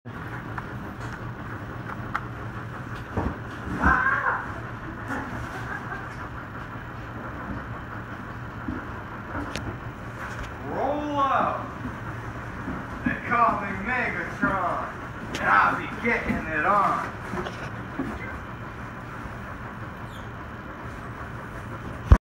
Transformers 7
Megatron has caused deviation to the city of metropolis. Casualties are high and Zach needs to find the all mighty cube to save the world from deviation. As tension rises and all hope seems lost a new hope arrives to save the day.